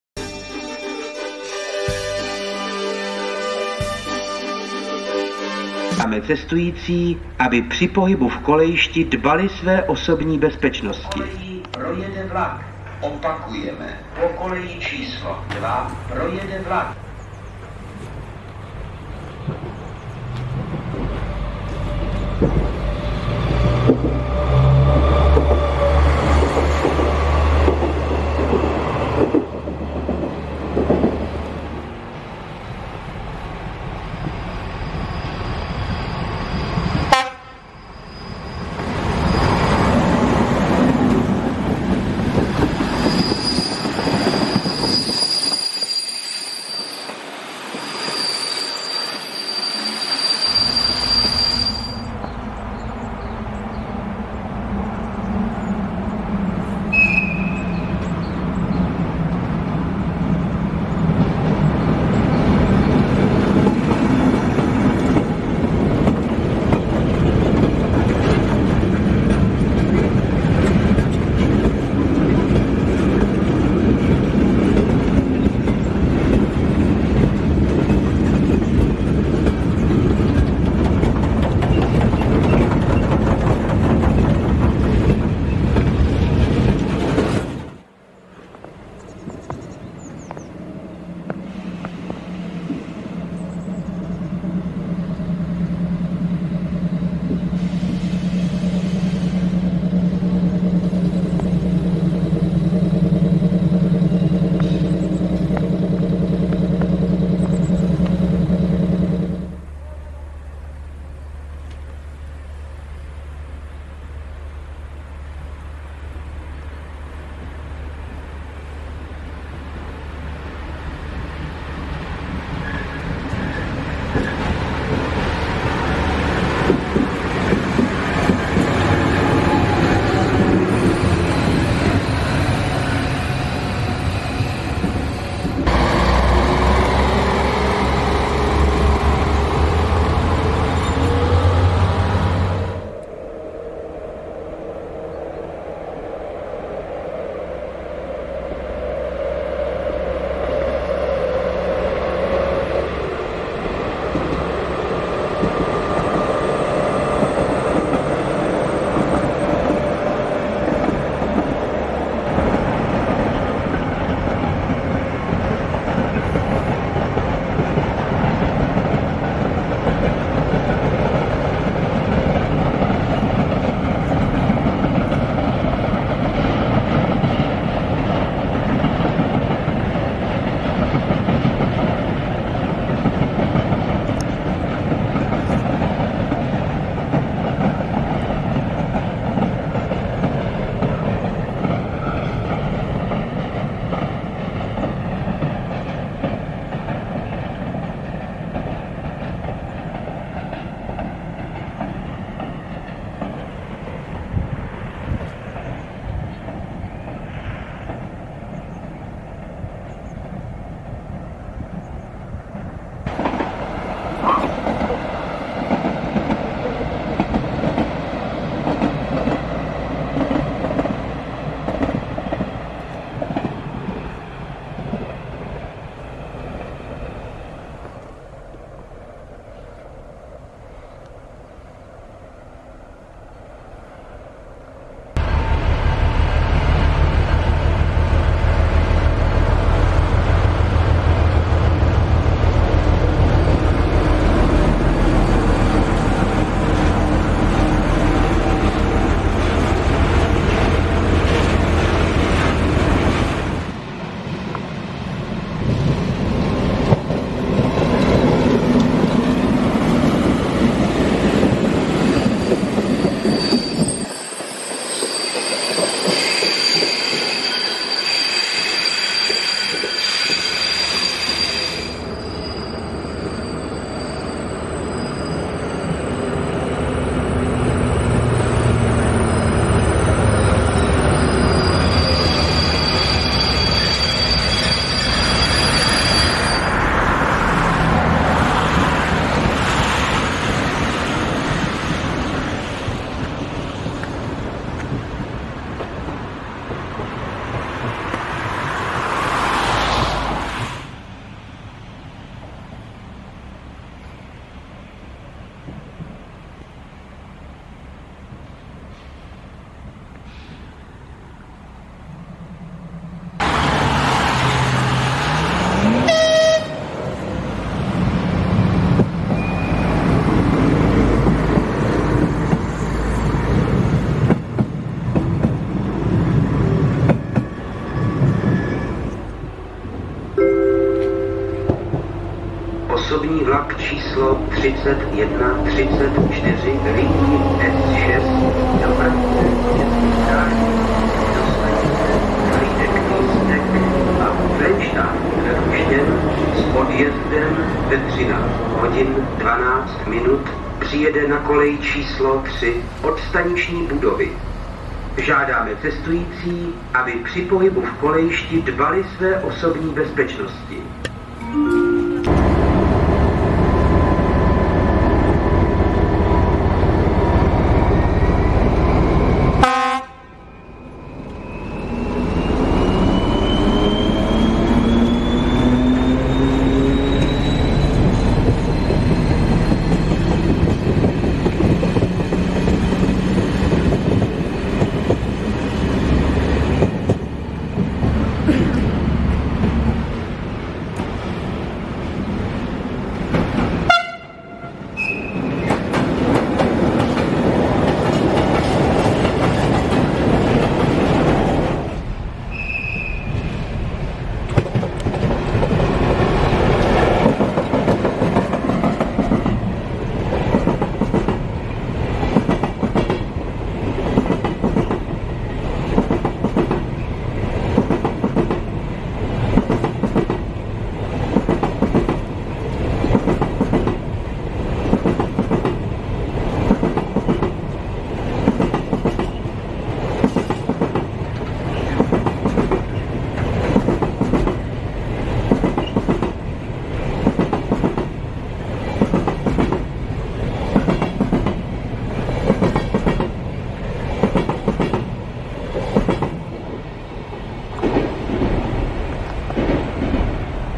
Vlaky PASKOV *** NEJLEPŠÍ ŠOCENÍ
ŠOCENÍ PASKOV - - - uvidíte:
00:00 Fanfára
00:06 Úvod
00:12 KVATRO - z peronu
00:31 BREJLOVEC - z úrovňového přechodu
00:52 BUCHAR - z konce nástupiště
01:27 KVATRO - z konce nástupiště
01:49 VESELÁ KRÁVA - z pilíře nadchodu
02:16 VESELÁ KRÁVA - z kolejiště
02:22 KVATRO - z kopce
02:42 BUCHAR - z kopce
03:35 KVATRO - z kopce
03:47 ČMELÁK - z kopce
03:56 ČMELÁK - z přejezdu
04:13 BREJLOVEC - z peronu
04:32 BREJLOVEC - z peronu
04:47 VESELÁ KRÁVA - z mostu
05:00 KVATRO - z mostu
05:29 KVATRO - z peronu
06:04 BREJLOVEC - z peronu
06:29 BUCHAR - z peronu
06:41 BATOHY - z kolejiště

SPOLUPRACOVALI *** Těšínský šotouš - Kachlyk cz - Masličko hh - (kjk studio)